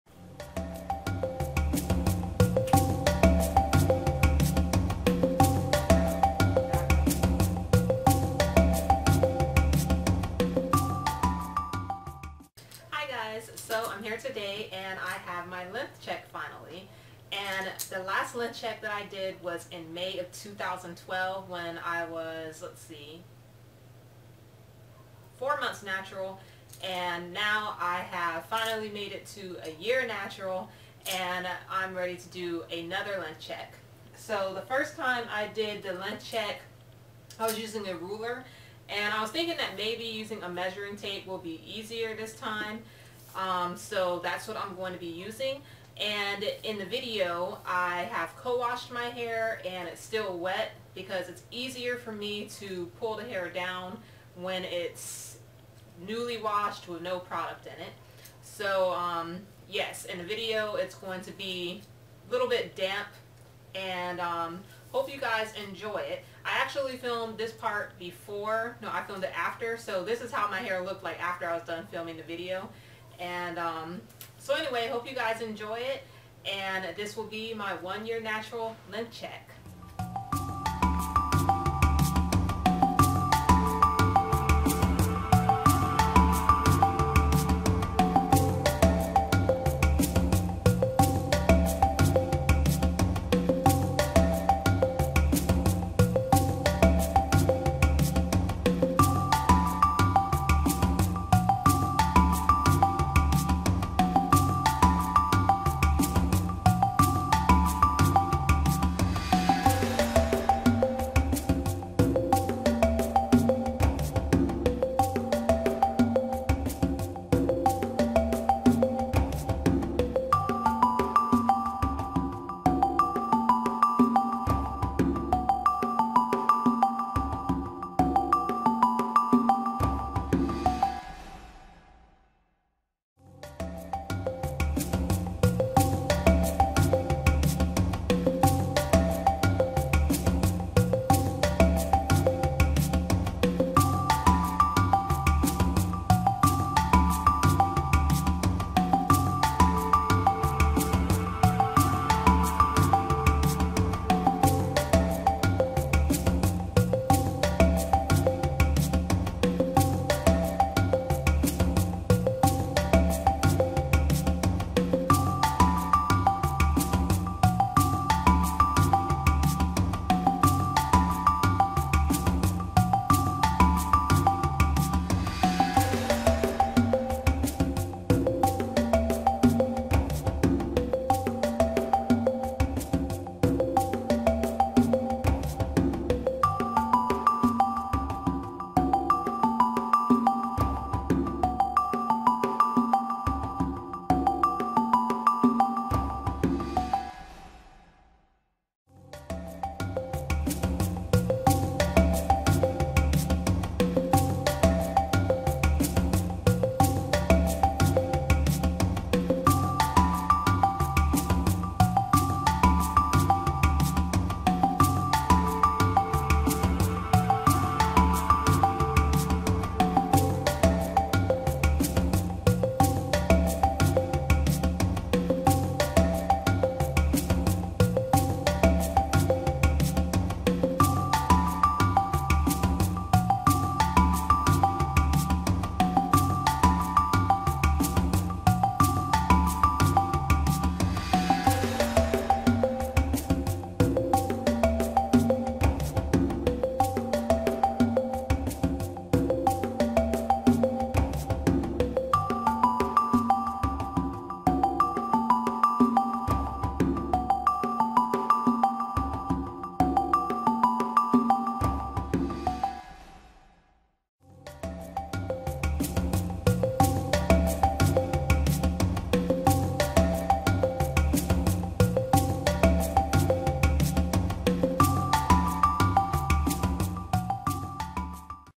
Natural Hair : Length Check #2 (1 YEAR NATURAL)
READ ME

Hi this is my length check at 1 year natural. My last length check was in May 2012 at 4 months natural.

There are more photos on my blog of my length check in HQ

The video is also available in HD

Thankks!